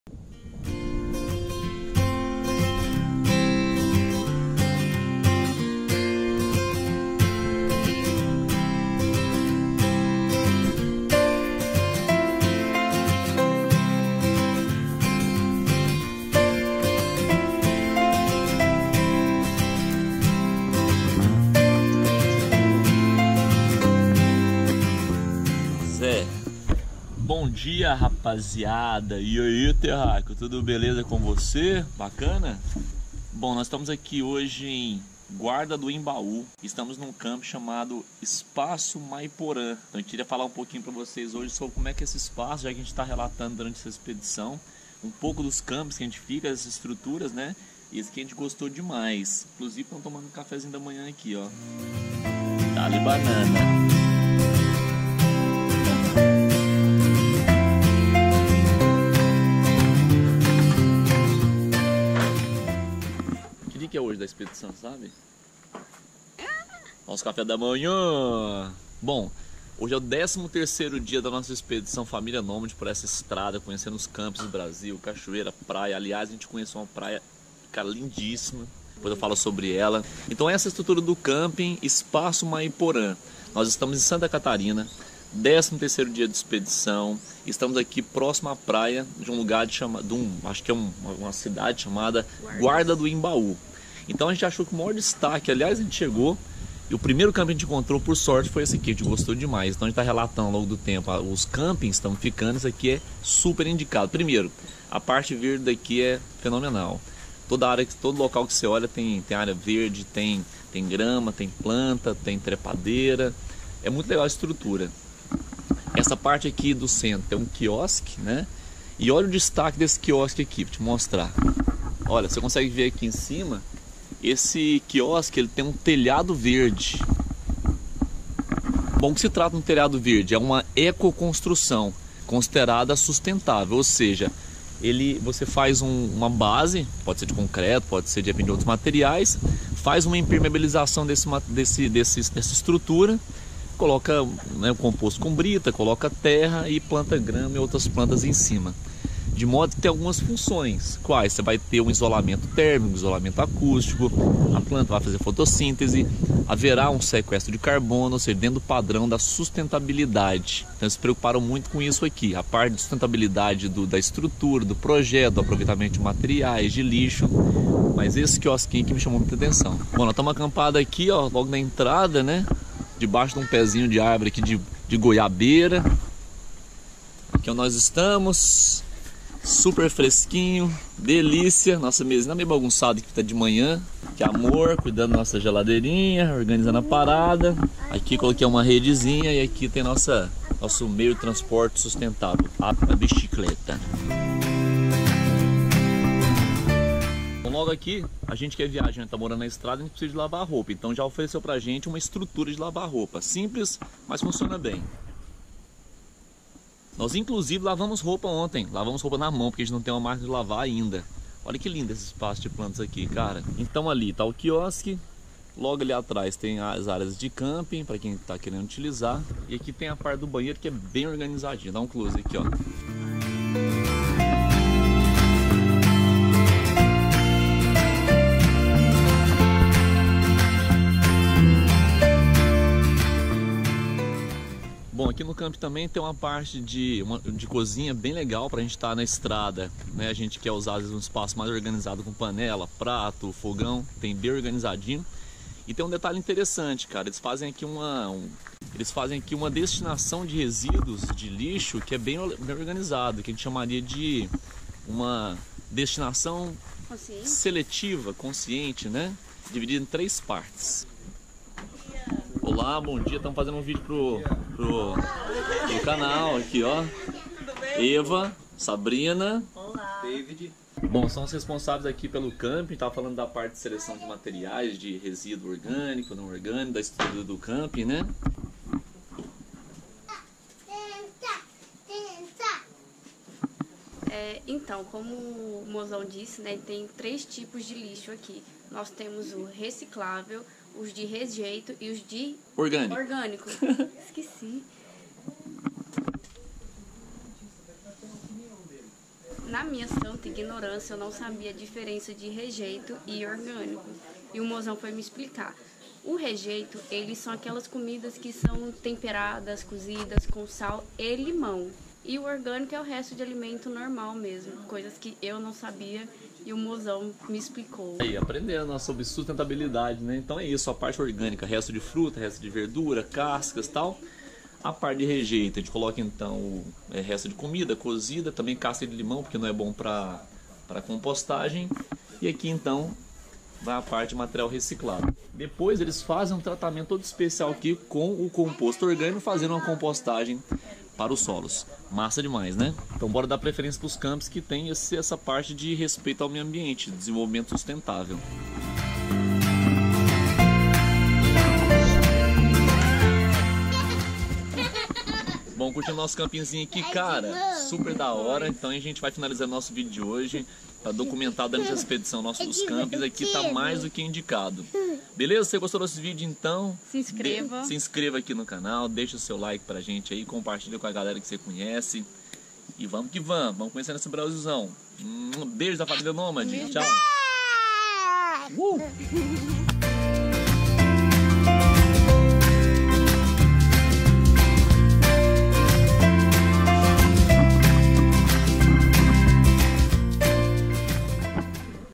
GUARDA DO EMBAÚ | SANTA CATARINA
Olha só que camping incrível. E o melhor: sustentável. Sim! Ficamos no camping Espaço Maeporã em Guarda do Embaú em Santa Catarina e ficamos muito satisfeitos. Lugar lindo, organizado e com uma pegada sustentável com disposição seletiva de lixo, telhado verde... adoramos!

COMO VIAJAR GASTANDO POUCO?

COMO GANHAR DINHEIRO NA INTERNET - CHORA OU VENDE LENÇOS